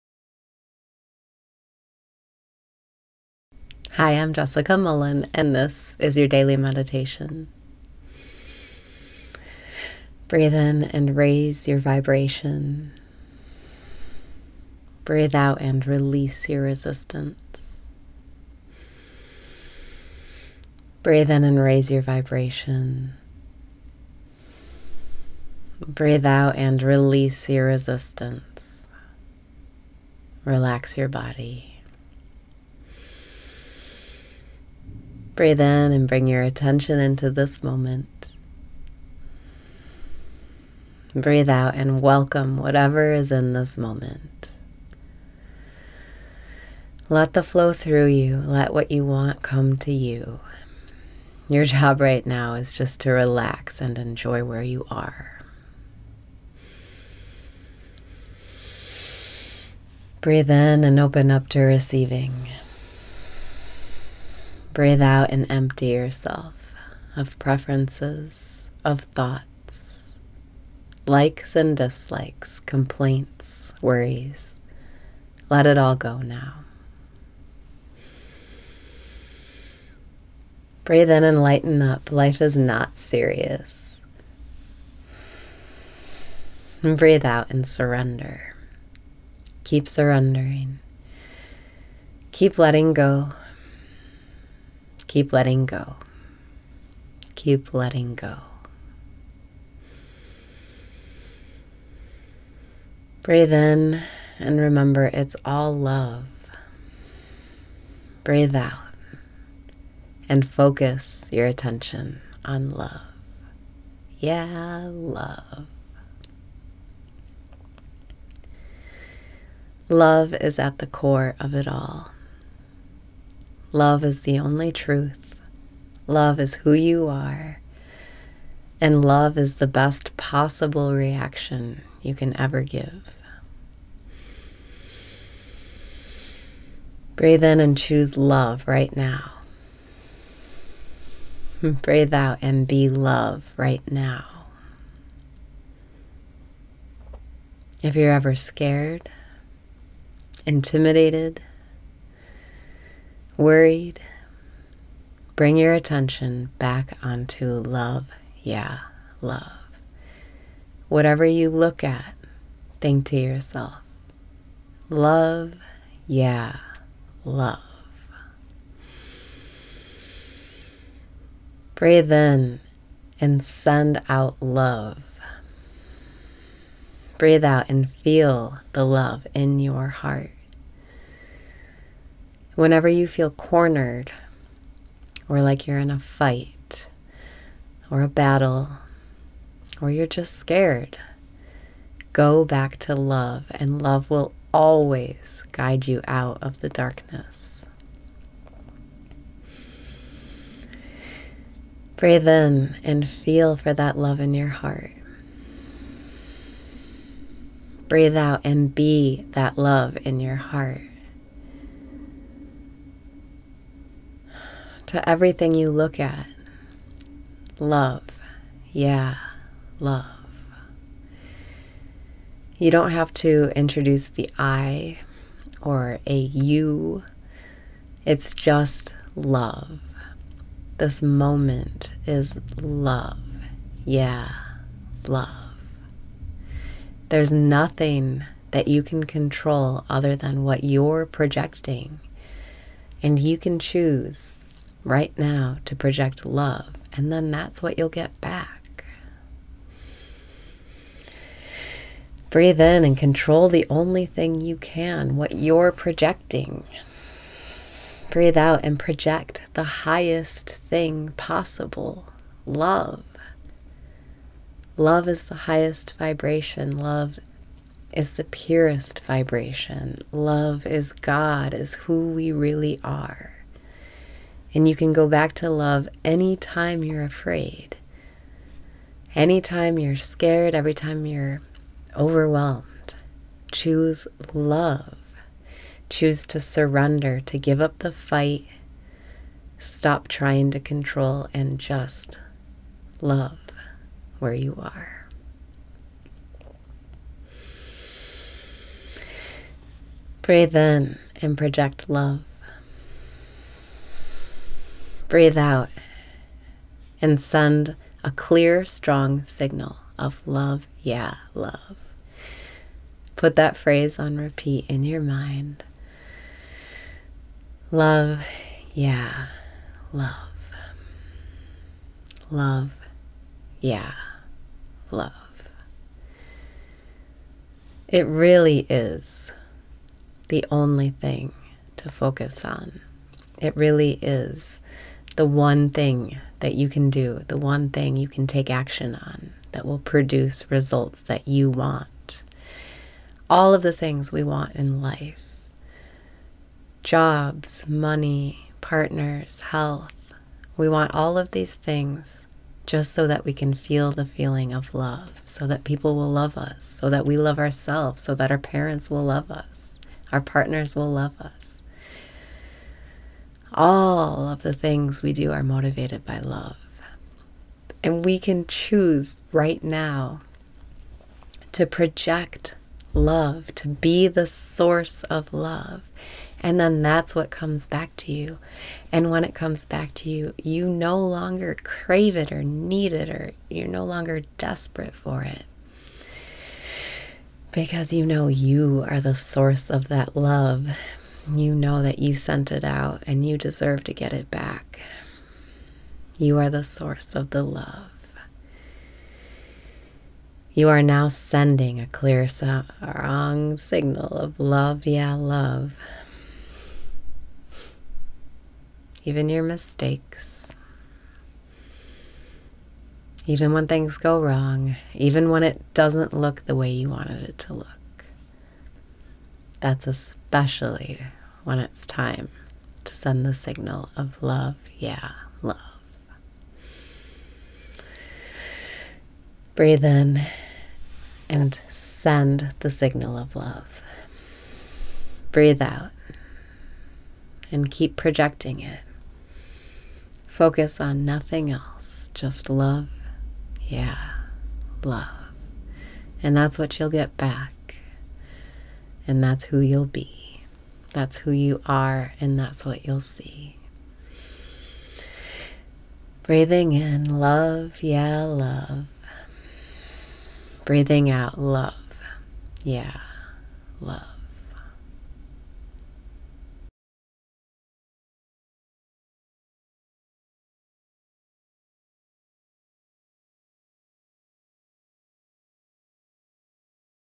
Daily Meditation - Give Up the Fight
Breathe in and bring your attention into this moment.
Breathe out and welcome whatever is in this moment.

Let the flow through you. Let what you want come to you. Your job right now is just to relax and enjoy where you are.

Breathe out and empty yourself of preferences, of thoughts, likes and dislikes, complaints, worries. Let it all go now.

Keep surrendering. Keep letting go. Keep letting go. Keep letting go.

Breathe in and remember it’s all love.
Breathe out and focus your attention on love, yeah, love.

Love is at the core of it all. Love is the only truth. Love is who you are. And love is the best possible reaction you can ever give.

Breathe in and choose love right now.
Breathe out and be love right now.

If you’re ever scared, intimidated, worried, bring your attention back onto love, yeah love. Whatever you look at, think to yourself, “Love, yeah, love!”

Breathe in and send out love.
Breathe out and feel the love in your heart.

Whenever you feel cornered, or like you’re in a fight, or a battle, or you’re just scared, go back to love and love will always guide you out of the darkness.

Breathe in and feel for that love in your heart.
Breathe out and be that love in your heart.

To everything you look at, “Love, yeah, love.” You don’t have to introduce the “I” or a “you”. It’s just love. This moment is love, yeah love! There’s nothing that you can control, other than what you’re projecting. And you can choose right now to project love and then that’s what you’ll get back.

Breathe in and control the only thing you can: what you’re projecting.
Breathe out and project the highest thing possible: love!

Love is the highest vibration. Love is the purest vibration. Love is god, is who we really are. And you can go back to love any time you’re afraid. Any time you’re scared, every time you’re overwhelmed. Choose love! Choose to surrender, to give up the fight, stop trying to control and just love where you are.

Breathe in and project love.
Breathe out and send a clear strong signal of love, yeah, love.

Put that phrase on repeat in your mind. Love, yeah, love. Love, yeah, love. It really is the only thing to focus on. It really is the one thing that you can do. The one thing you can take action on, that will produce results that you want. All of the things we want in life—jobs, money, partners, health—we want all of these things, just so we can feel the feeling love. So that people will love us. So that we love ourselves. So that our parents will love us, our partners will love us.

All of the things we do are motivated by love. And we can choose right now to project love. To be the source of love. And then that’s what comes back to you. And when it comes back to you, you no longer crave it or need it—you’re no longer desperate for it. Because you know YOU are the source of that love. You know that you sent it out and you deserve to get it back. You are the source of the love. You are now sending a clear strong signal of love, yeah, love. Even your mistakes. Even when things go wrong. Even when it doesn’t look the way you wanted it to look. That’s especially when it’s time to send the signal of love, yeah love.

Breathe in and send the signal of love.
Breathe out and keep projecting it. Focus on nothing else. Just love, yeah love. And that’s what you’ll get back and that’s who you’ll be. That’s who you are. And that’s what you’ll see.

Breathing in love, yeah love.
Breathing out love, yeah love.